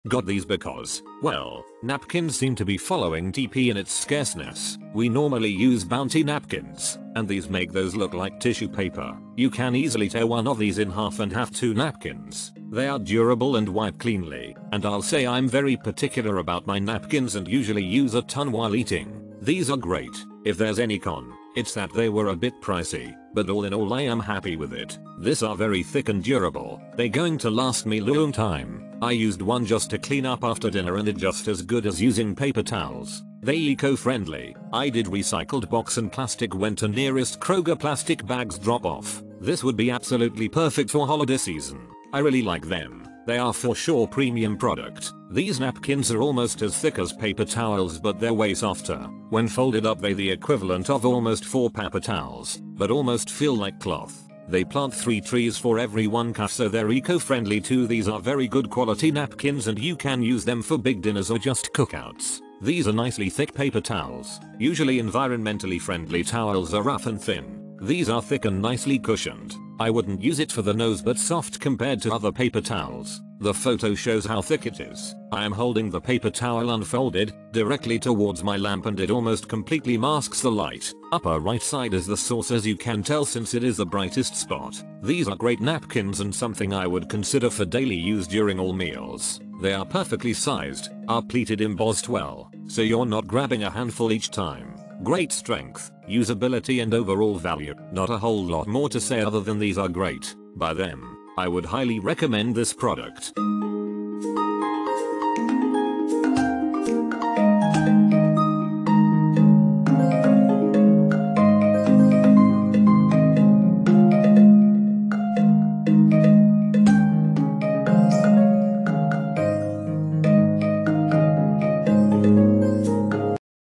Aria Premium Earth Friendly Napkins, 960-Count 2-Ply Napkins, 12 Packs of 80, review
Main Features:
Aria Napkins combine sustainable manufacturing with a soft, strong, cloth-like texture.. Make the eco-friendly choice with Aria’s innovative 94% plant-based, recyclable packaging.. Aria is made with responsibly sourced trees, and we plant 3 new trees for each one used.. Aria Napkins are thick and absorbent—perfect for everyday meals and snacks!. 12 packs with 80 napkins each (960 total napkins) of Aria Premium Earth-Friendly Napkins. 12 packs of 80 NAPKINS, 2-PLY, 1,981.2 SQ IN (1,327.8 dm2), 12.9 IN X 12.8 IN (32.7 cm X 32.5cm)